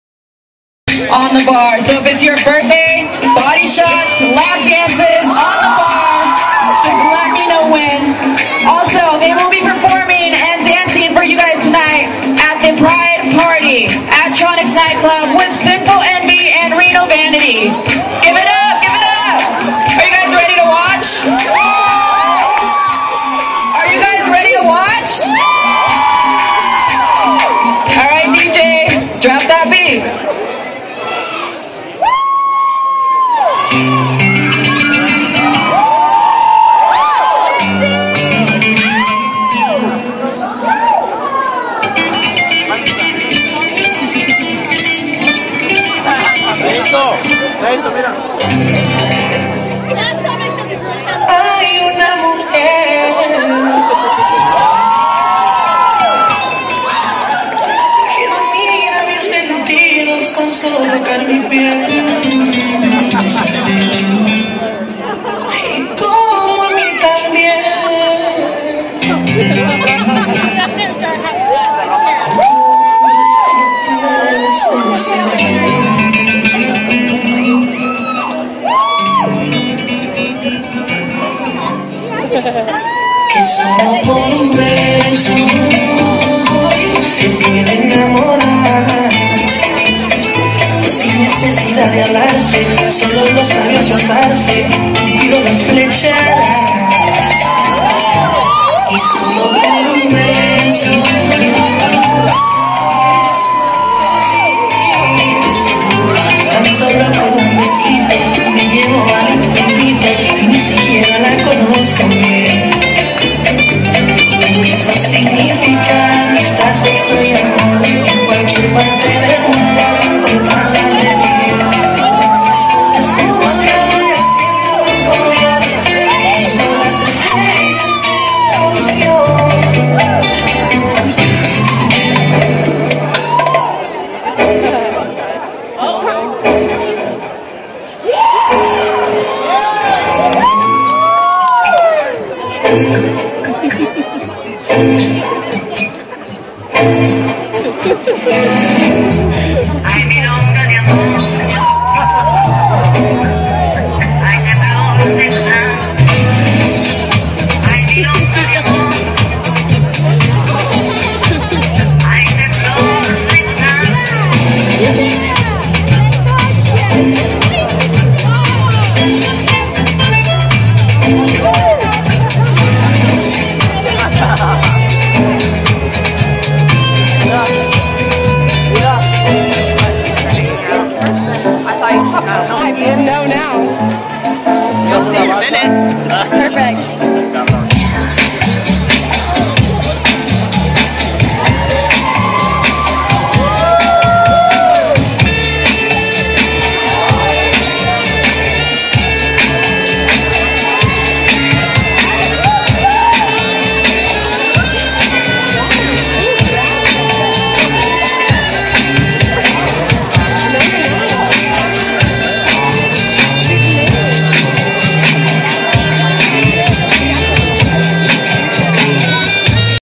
Sinful Femmes and Studs couples dance Reno Pride
Sinful NV Femmes and Studs bring you a sexy couples dance in the Latina section of gay pride on the Wingfield Park stage in Reno, NV.